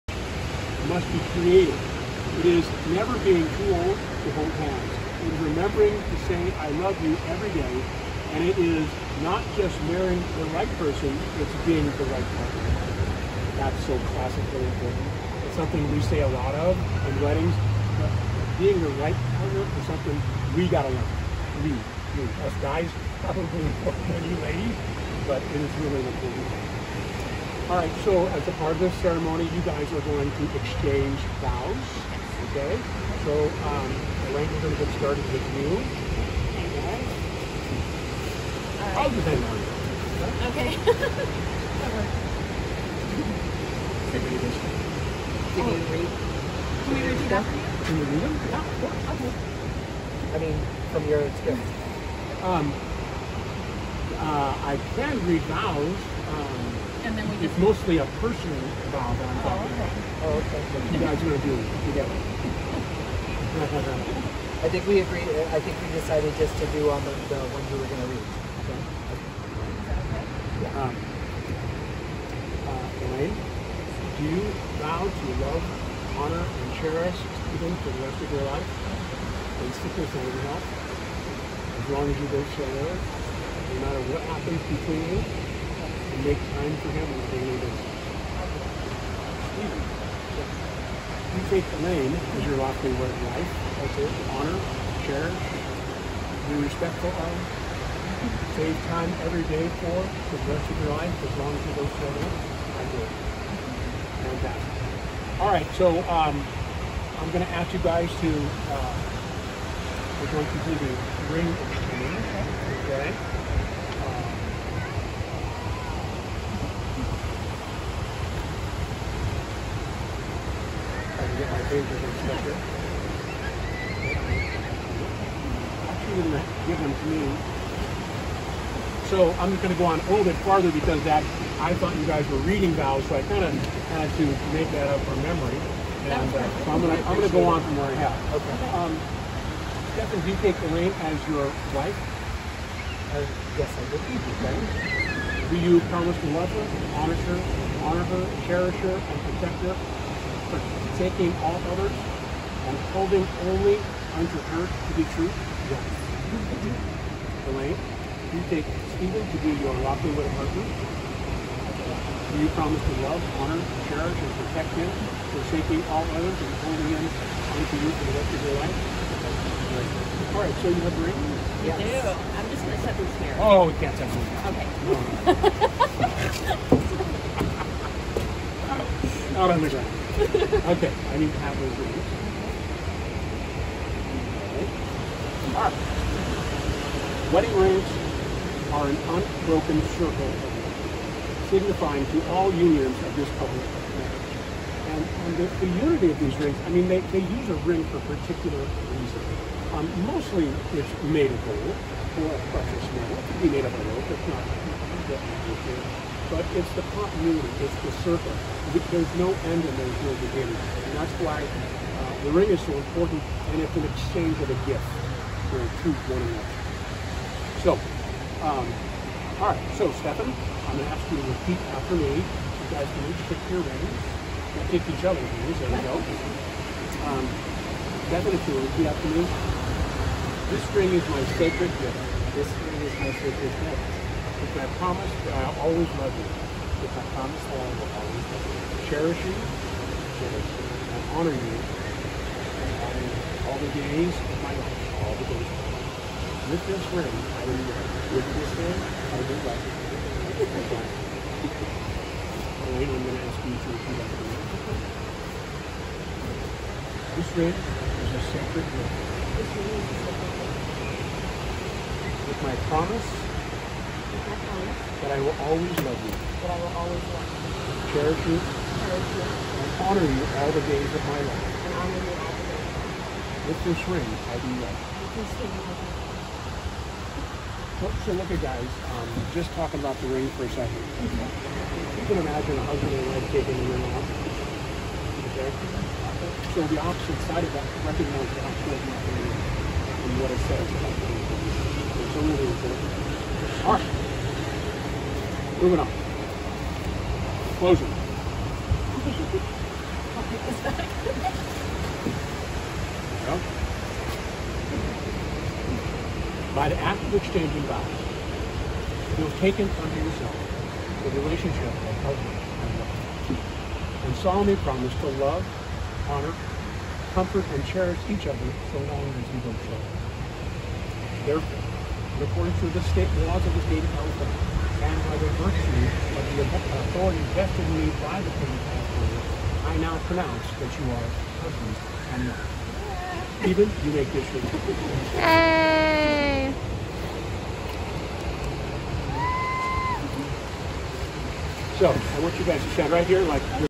Stephan and Elaine's COVID 19 Ventura Pier Beach Wedding
August 1, 2020 11:00 AM, Ventura California. Stephan and Elaine were married by Cliff Housego on Life Guard Station #10 next to the Ventura Pier.